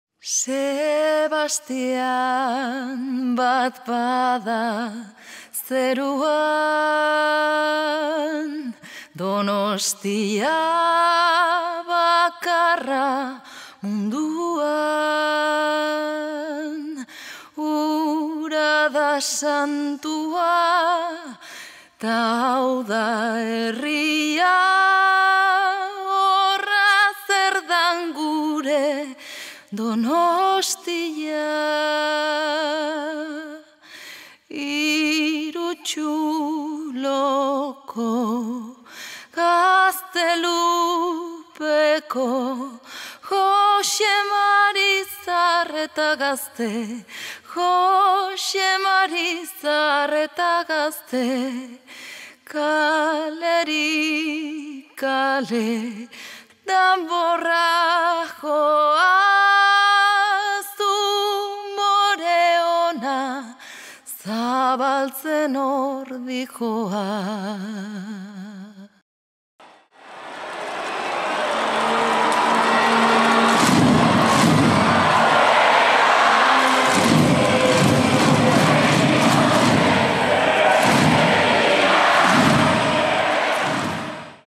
Gora Donostia 2017 │Eneko Goia
GORA DONOSTIA!

Milaka une. Milaka oroitzapen. Baina bat bakarra munduan. Ondo pasa San Sebastián Eguna!

Miles de momentos. Miles de recuerdos. Bat bakarra munduan. ¡Disfruta del día de San Sebastián!

Eneko Goia
Donostiako Alkatea